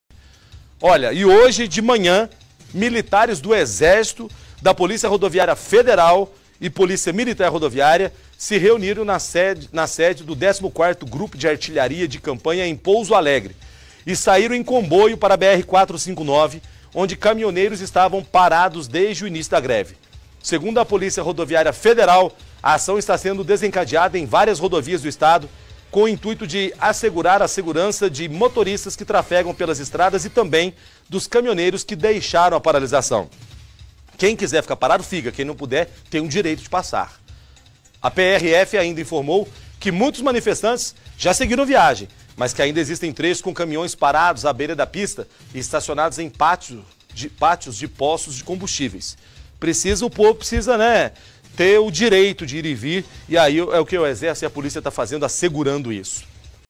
Exército faz a segurança em estradas da região
E, hoje de manhã, militares do exército, da polícia rodoviária federal e polícia militar rodoviária se reuniram na sede do 14º grupo de Artilharia de Campanha, em Pouso Alegre, e saíram em comboio para a BR-459, onde caminhoneiros estavam parados desde o início da greve. Segundo a polícia rodoviária federal, a ação está sendo desencadeada em várias rodovias do estado com o intuito de assegurar a segurança de motoristas que trafegam pelas estradas e, também, dos caminhoneiros que deixaram a paralisação. A prf informou que muitos manifestantes já seguiram viagem, mas que ainda existem trechos com caminhões parados à beira da pista e estacionados em patios de postos de combustíveis.